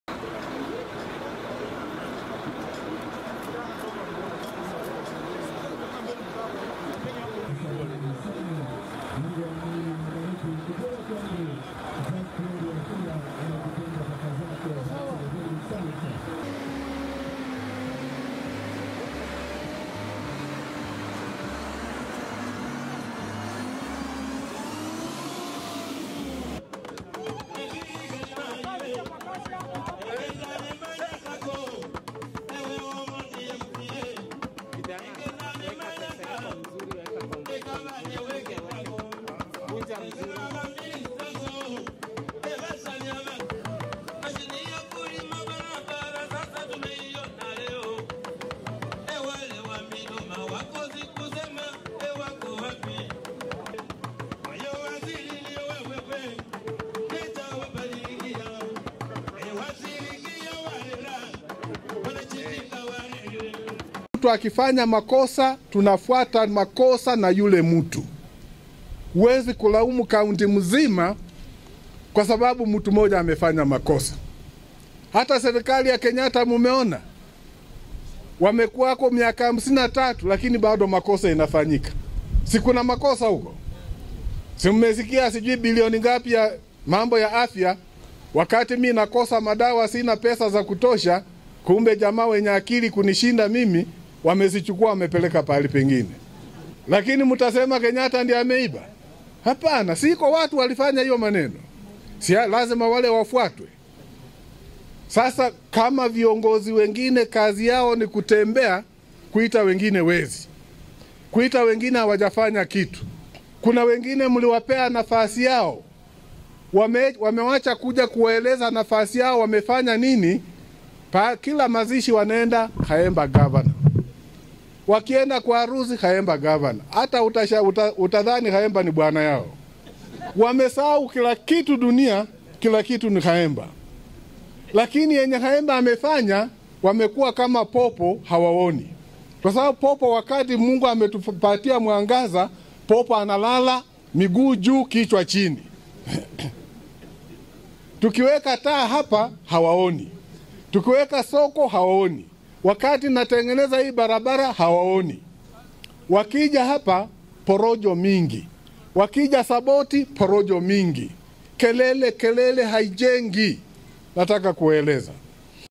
Trans Nzoia Governor wants the war on corruption to be zereod in to implicated individuals
Trans Nzoia Governor Patrick Khaemba wants the war on corruption to be zeroed in to individuals instead of launching blanket blames on institutions where they are stationed in.
Speaking at Saboti, governor Khaemba said the war on graft will be far from being won if focus will be centered on naming and shaming institutions where alleged corrupt officials are stationed.
The Governor said he will no longer carry the cross on alleged cases on graft in his government and instead said any officer who will be named in any scam will be held accountable personally.